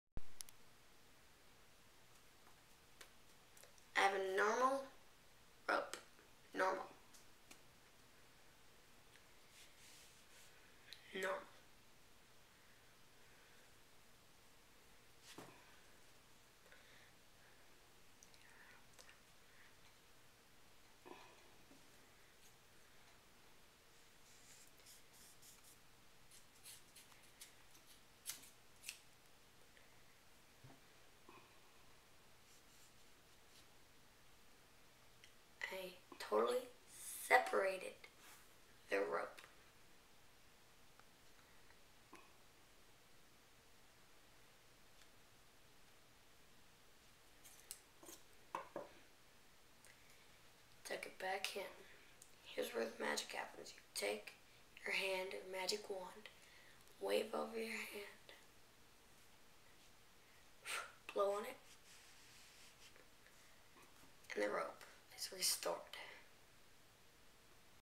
cut rope trick from Luke Loves Homemade Magic.com
Amazing trick on cutting a rope in two and then restoring it.